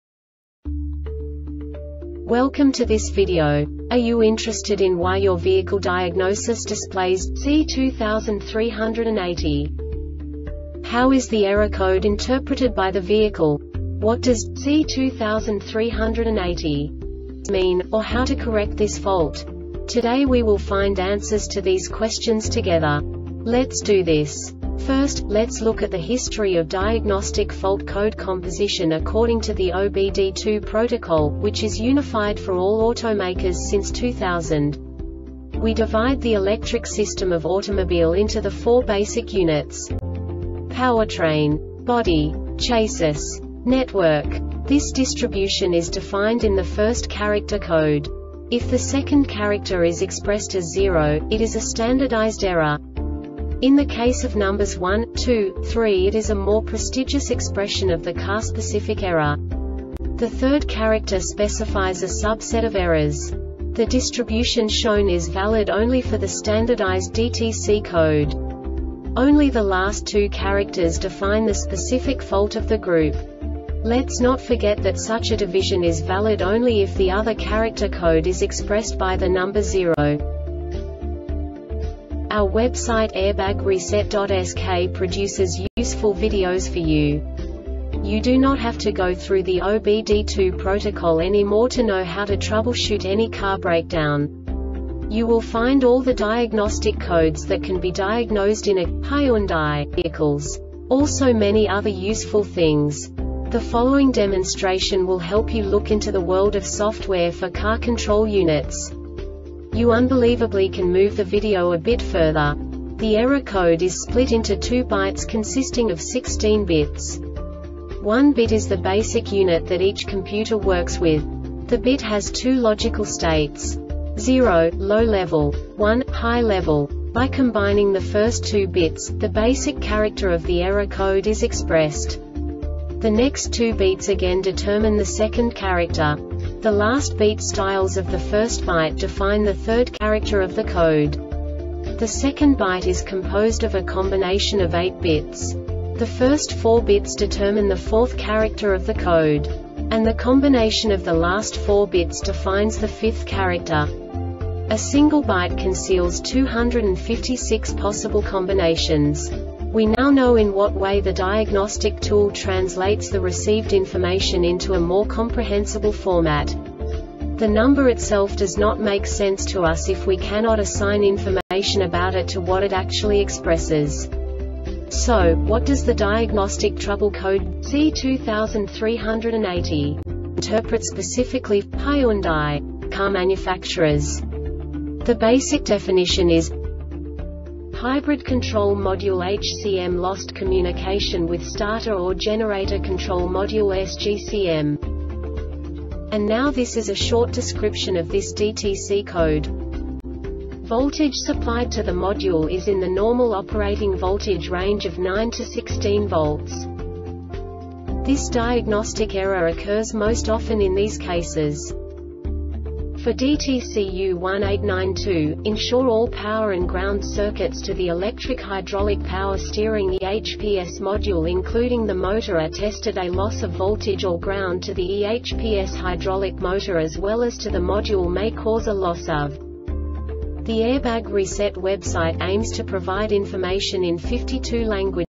DTC Hyundai C2380 Short Explanation
Contents:
0:21 Basic DTC analysis according to OBD2 protocol standard.
1:48 Insight into programming
2:58 A diagnostically understandable description of the Diagnostic Trouble Code
3:05 Basic error definition

Make:
Hyundai
Code:
C2380
Definition:
Hybrid Control Module (HCM) Lost Communication With Starter or Generator Control Module (SGCM)
Description: